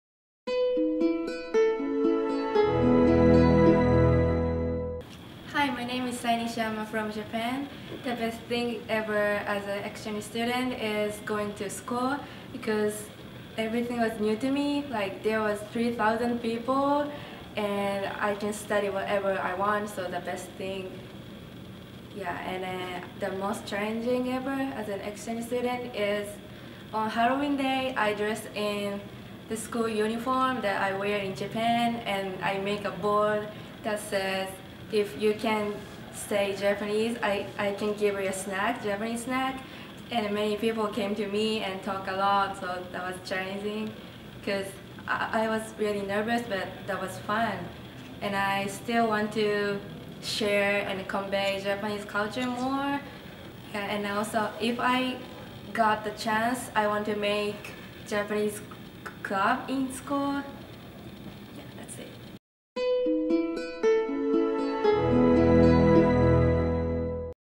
"Best, Worst, Next” - Video 6 of 12 - Sae from Japan
Sae from Japan shares (1) her best moment as an exchange student, (2) the most challenging part so far, and (3) what she's still looking forward to.